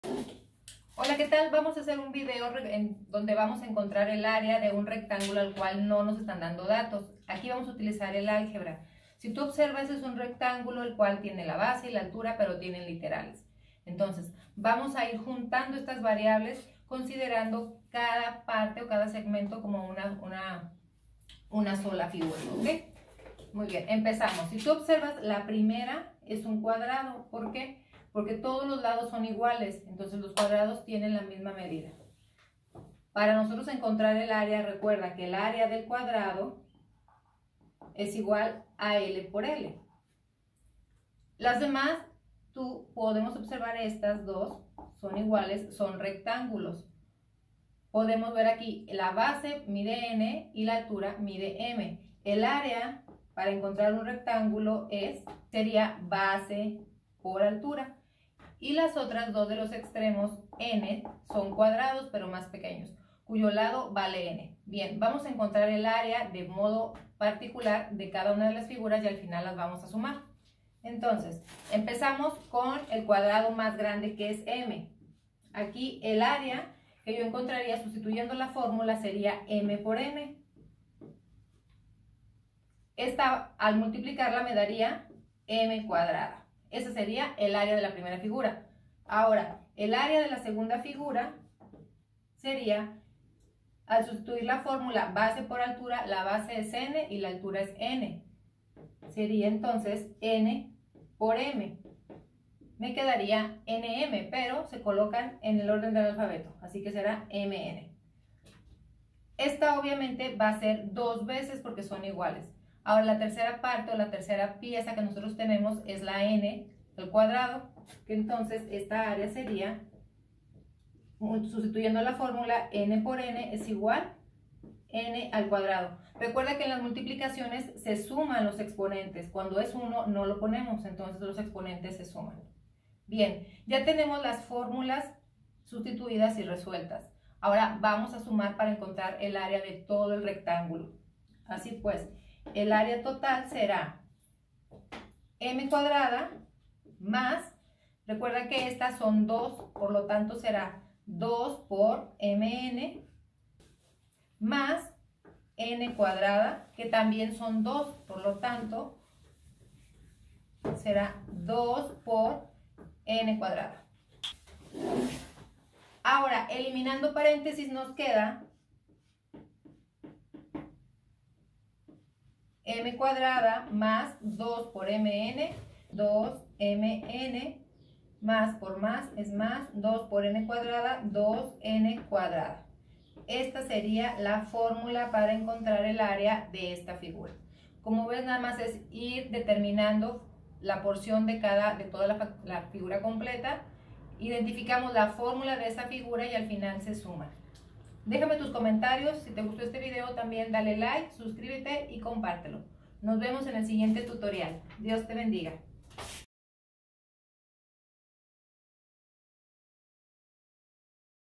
Área de rectángulo con álgebra
Encontrar la fórmula para el área de una figura con álgebra